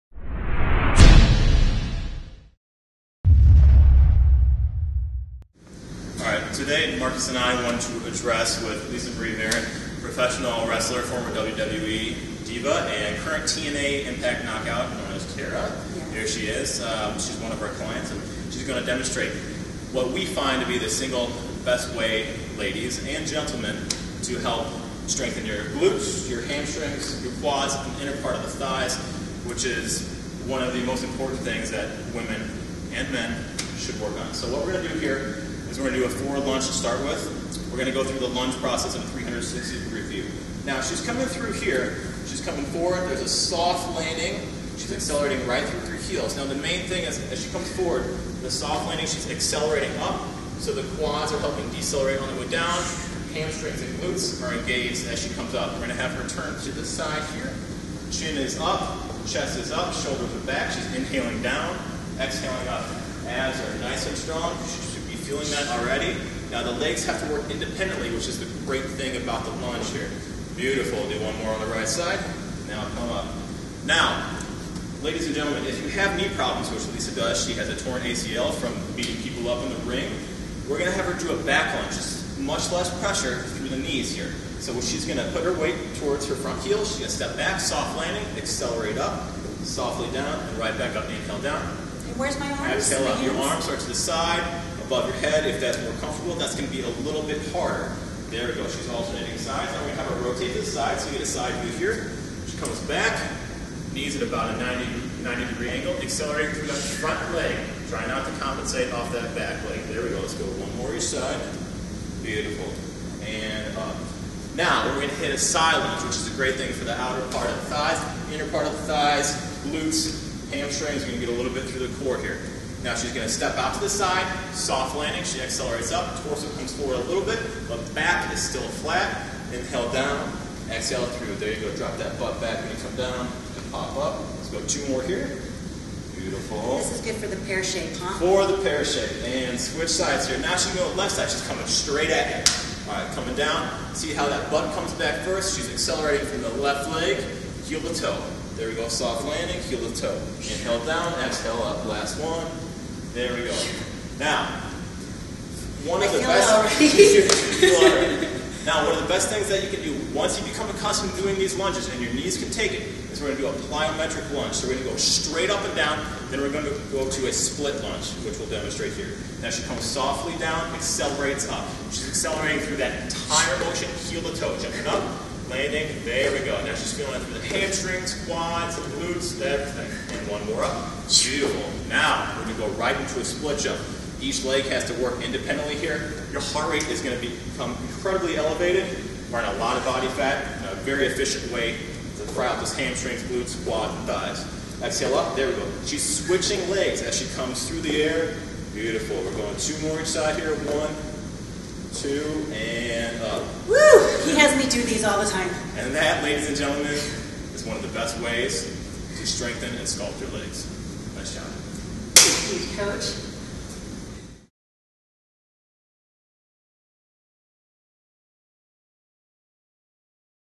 Lunge Right
Chicago Trainers and owners of Morph Personal Training LLC, Joey Thurman (WBFF Fitness Model Competitor) and Marcus Warren (Mr. Natural Australia 2001) This video features Lisa Marie Varon current pro wrestler with TNA Impact Wrestling Tara, and former WWE Diva Victoria! Watch as Lisa demonstrates and Joey describes a proper lunge and progressions of the lunge form the most basic to most difficult.